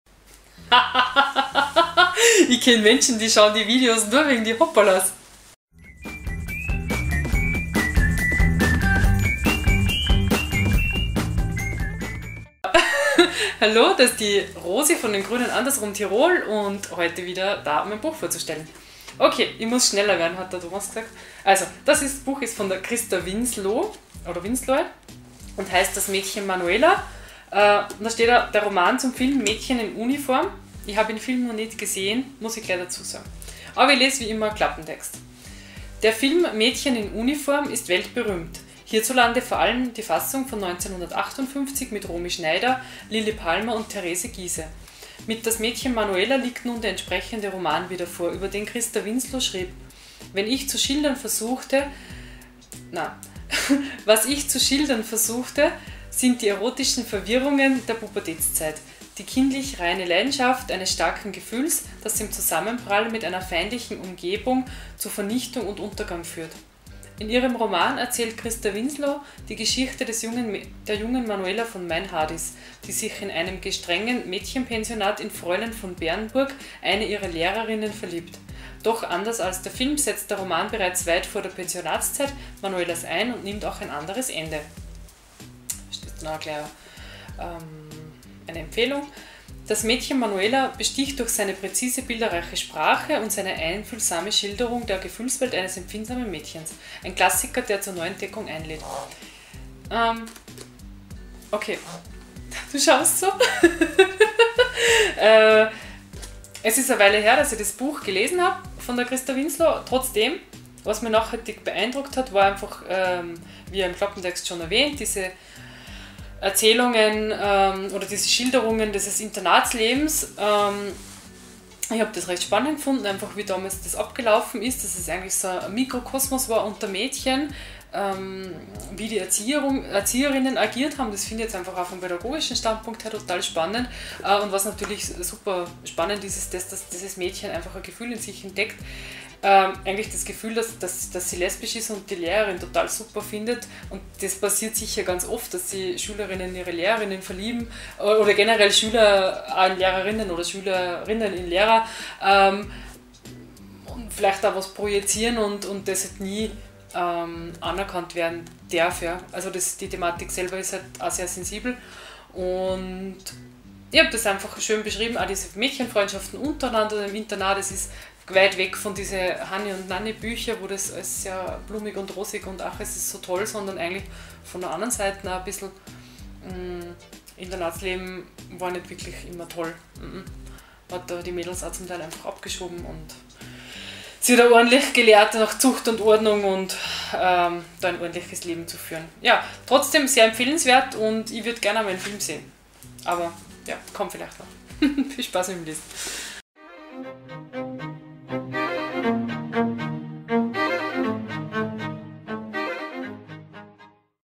Rosi's Buchtipp #28
~ Das Mädchen Manuela ~
von Christa Winsloe

Der Film Mädchen in Uniform ist weltberühmt - vor allem die dritte Fassung von 1958 mit Romy Schneider, Lilli Palmer und Therese Giehse. Mit Das Mädchen Manuela liegt nun der entsprechende Roman wieder vor, über den die Autorin Christa Winsloe schrieb: "Was ich zu schildern versuche, sind die erotischen Verwirrungen der Pubertätszeit, die kindlich reine Leidenschaft eines starken Gefühls, das im Zusammenprall mit einer feindlichen Umgebung zu Vernichtung und Untergang führt." Den Stoff zu ihrem Roman lieferten Winsloe ihre eigenen Erlebnisse aus ihrer Zeit in einem Potsdamer Mädchenpensionat: Sie erzählt die tragische Geschichte einer Schülerin, die sich hoffnungslos in ihre Lehrerin verliebt und sich das Leben nimmt, nachdem sie von ihr zurückgewiesen wurde.